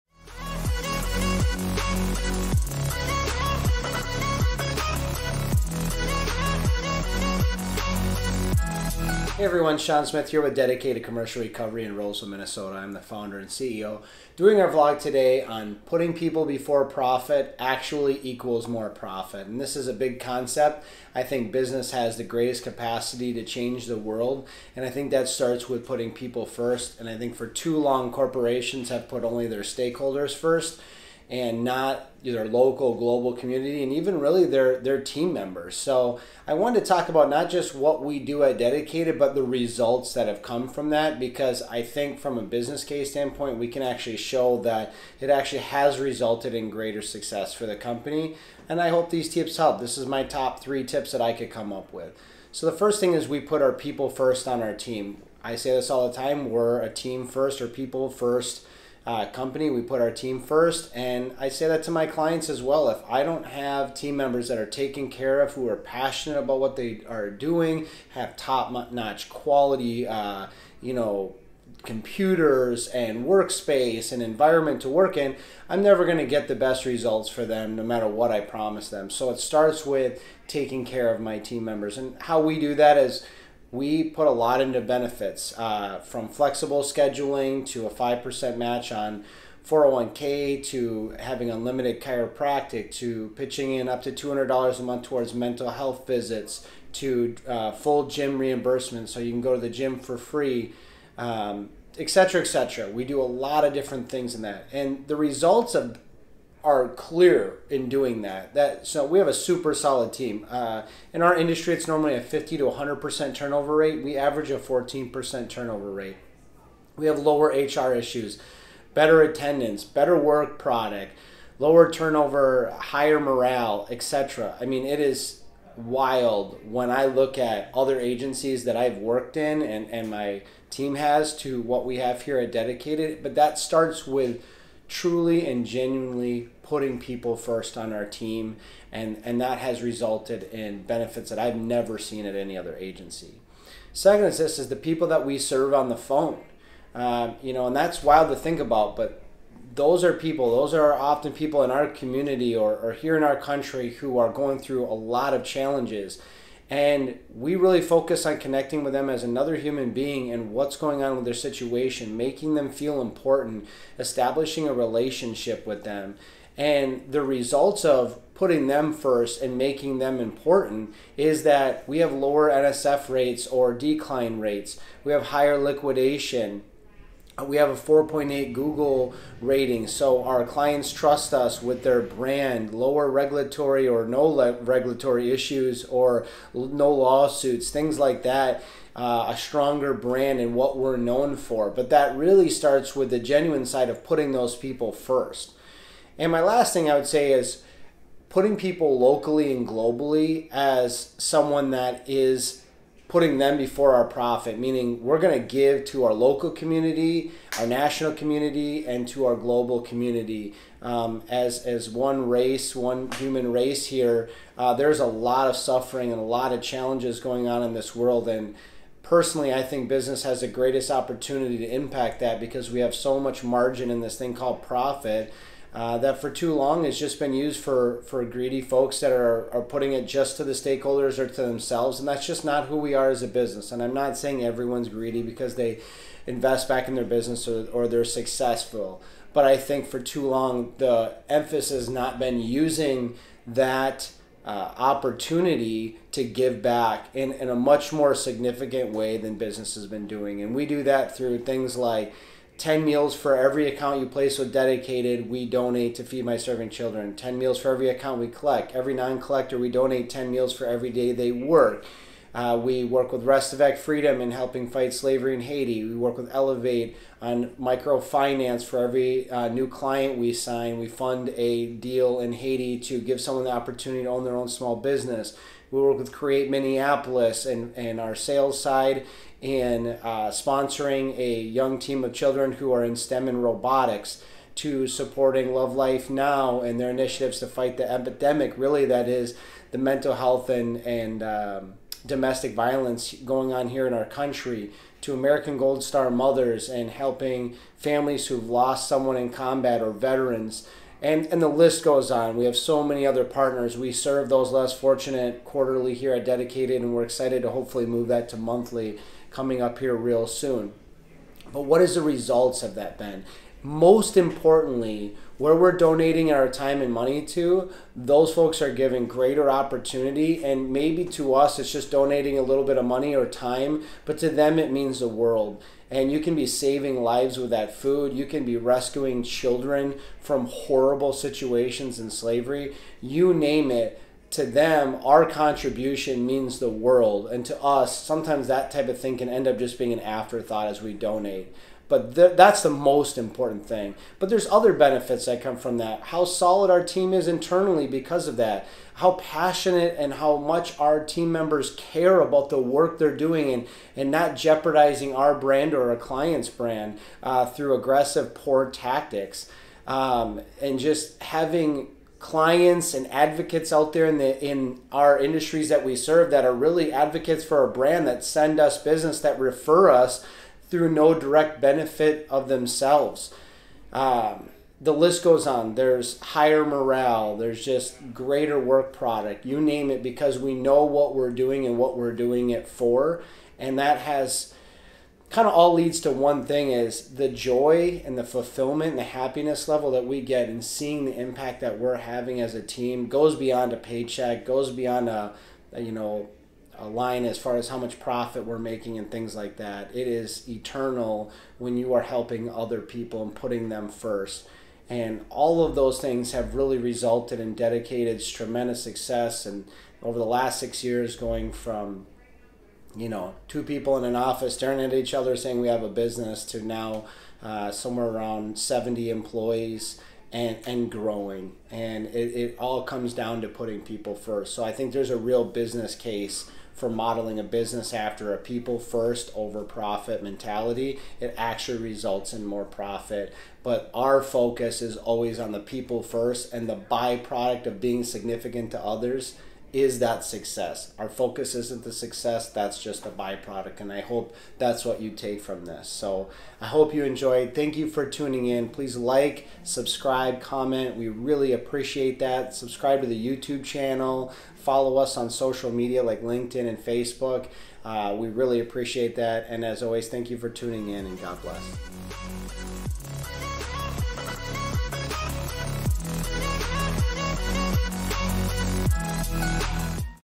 Putting people before profit = more profit. Vlog #29
At Dedicated, we believe that putting people before profit actually leads to more profit. Shawn gives his Top 3 tips on how to put people first and discover the undeniable results that doing this can bring.